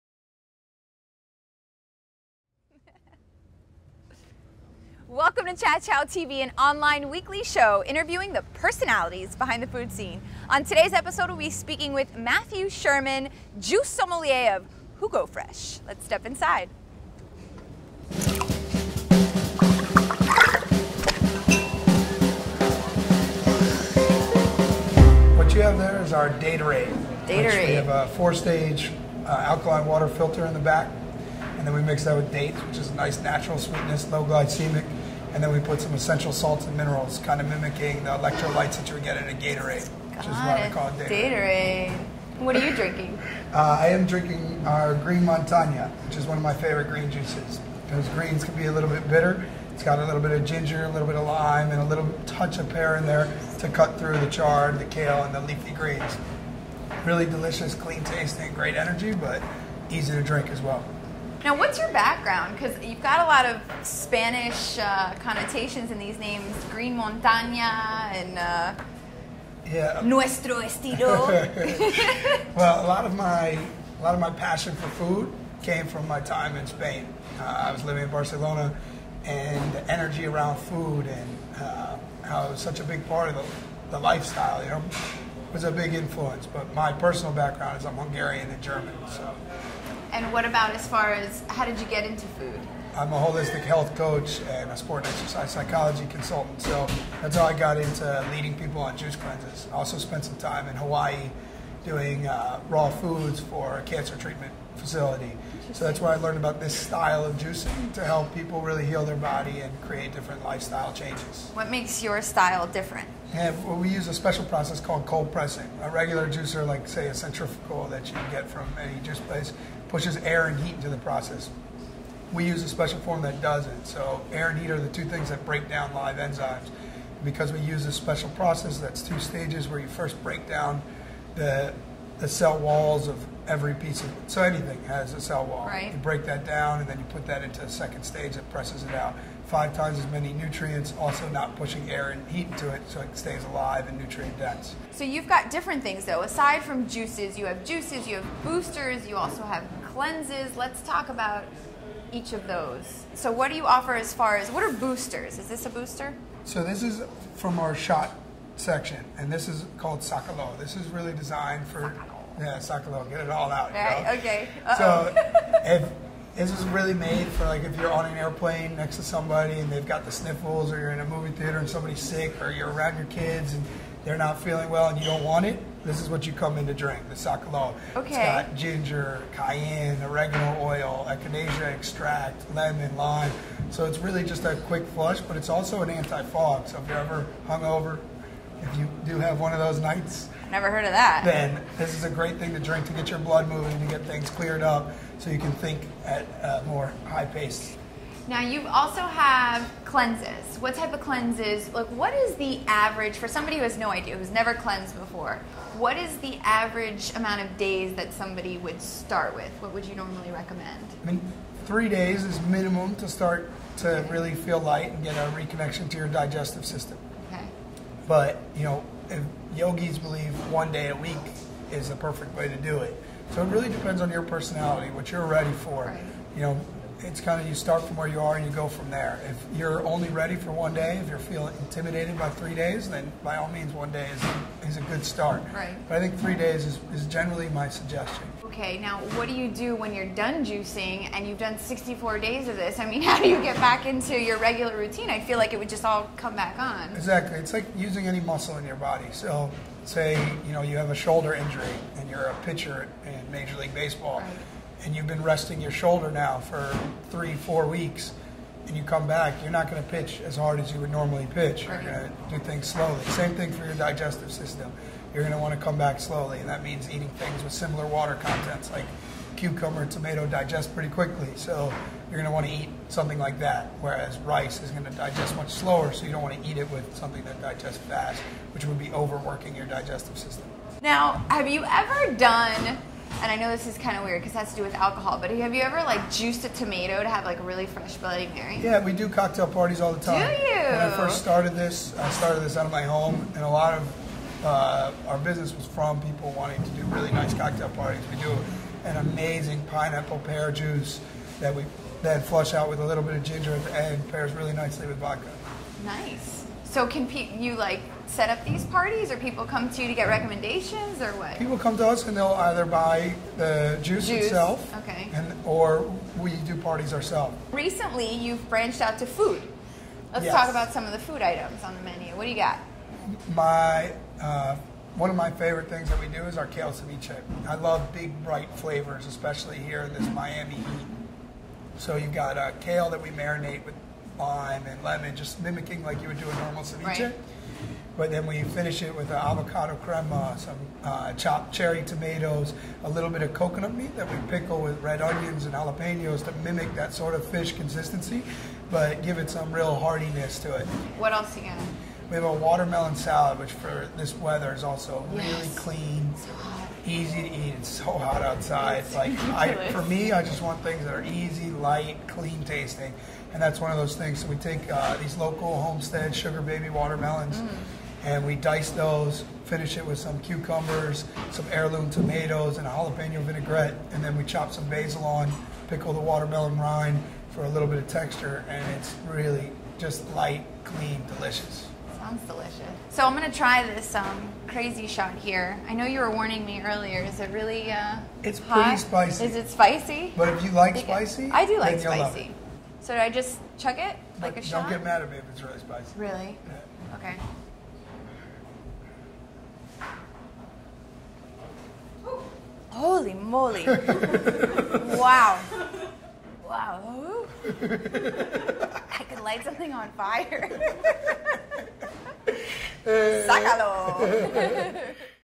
Matthew Sherman / Jugo Fresh
JugoFresh is bringing a fresh and authentic new concept to Miami Beach that will get locals thinking outside the "juice box". The new 870 sqft. juice bar, located at 1935 West Avenue (20th Street between West Avenue and Alton Road), in the emerging Sunset Harbour neighborhood, features more than 28 cold-pressed, alive juices pressed from organic fruits and vegetables.